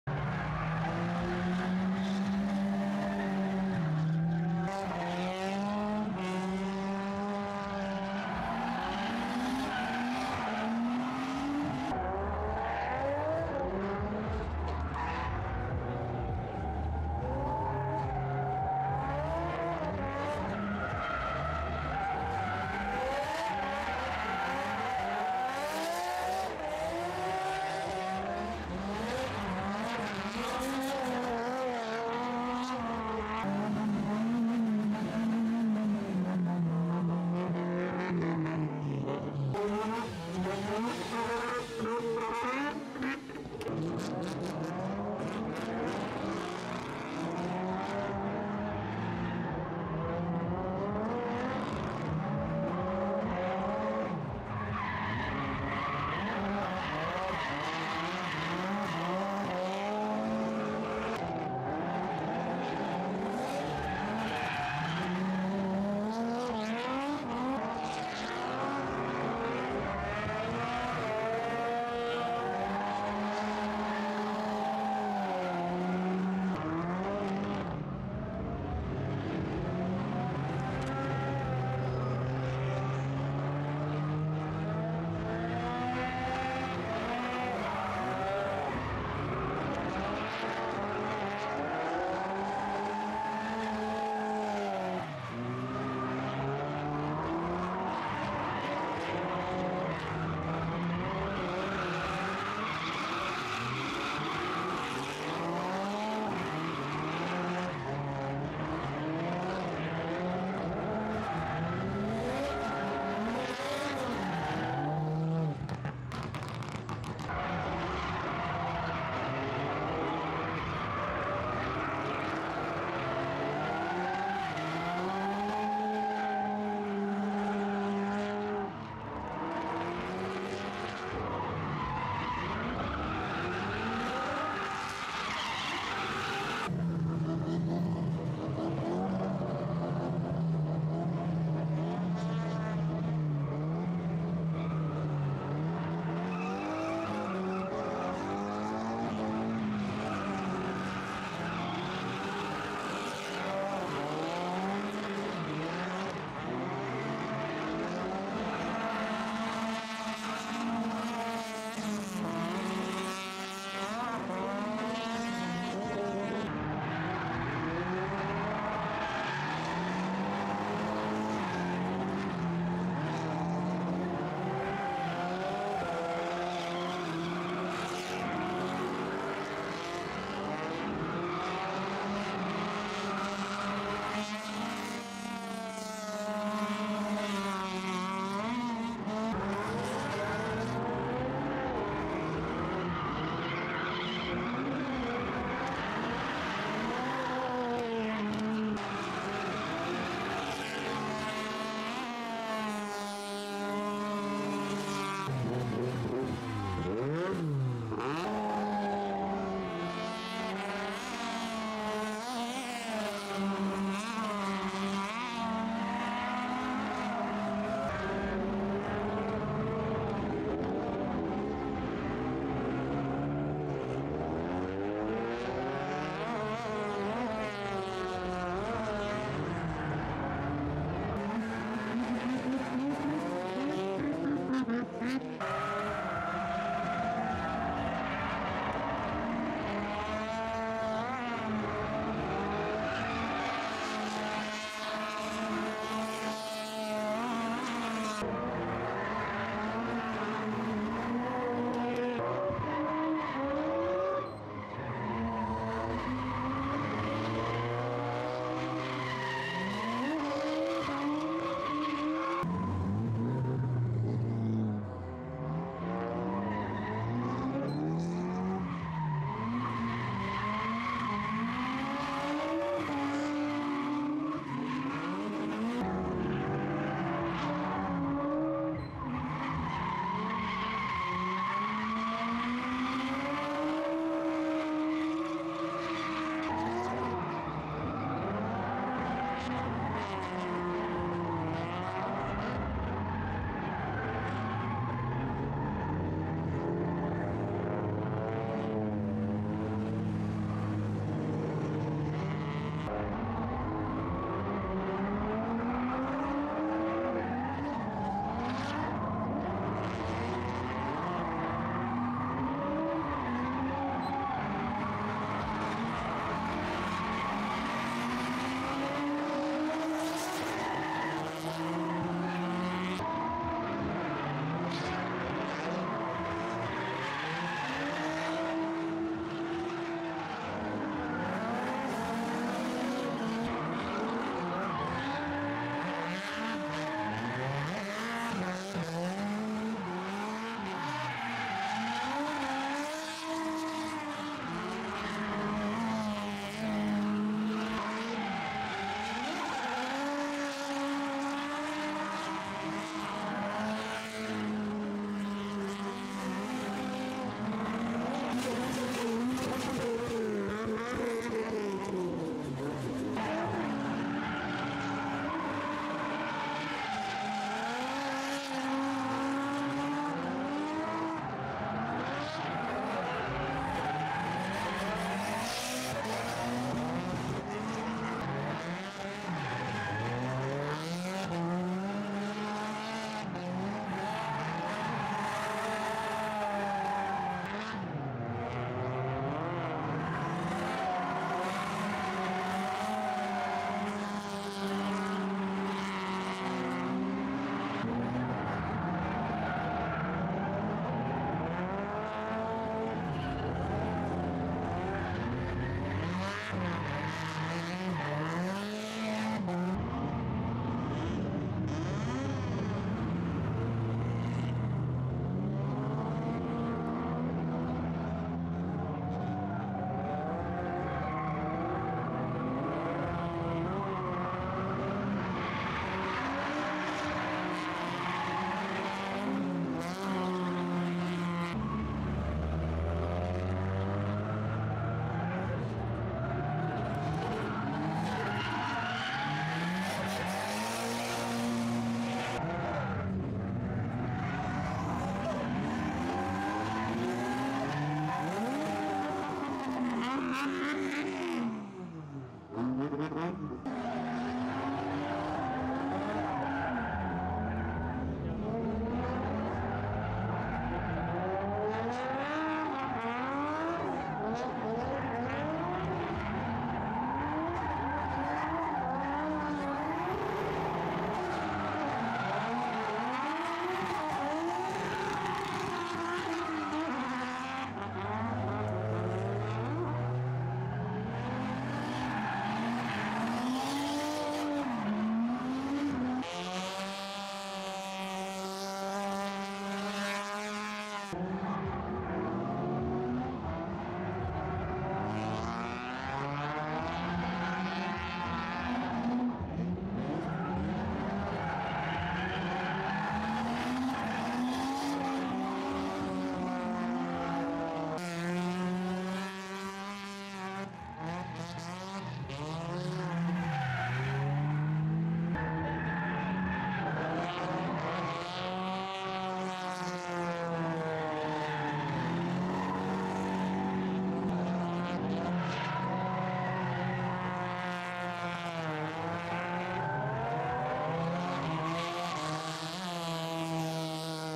DORIDORE 2023 Drifting event | JDM | USDM | JAPAN
OKUIBUKI MOTOR  PARK / SHIGA, JAPAN

DORIDORE is a car meeting event that means "DRIft" and "DREss up".
Drift runs are held on the lower floor, while customized cars are displayed (Fenderist) on the upper floor. They were mostly amateurs, but they were very good at drifting.